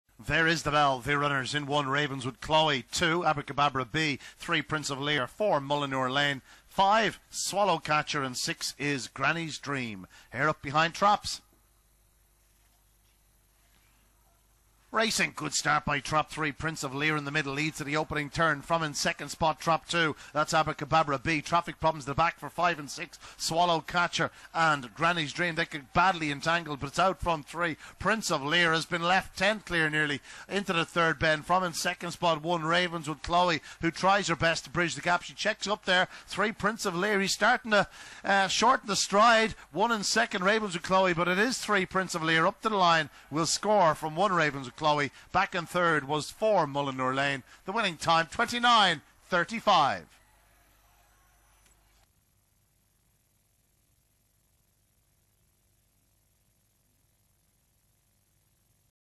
IGB - The Try A Trio A6 Semi-Final 30/10/2018 Race 3 - Shelbourne Park
Race: The Try A Trio A6  Semi-Final
Track Name: Shelbourne Park
Date: 30/10/2018
Hour: 20:10

Results

1st    3     PRINCE OF LIR       3/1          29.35
2nd    1     RAVENSWOOD CHLOE    4/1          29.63
3rd    4     MULLINURE LANE      6/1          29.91
4th    6     GRANNYS DREAM       7/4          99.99
5th    5     SWALLOW CATCHER     4/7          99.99
6th    2     ABRAKEBABRA BEE     6/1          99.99

|SPK_2018_10_30_03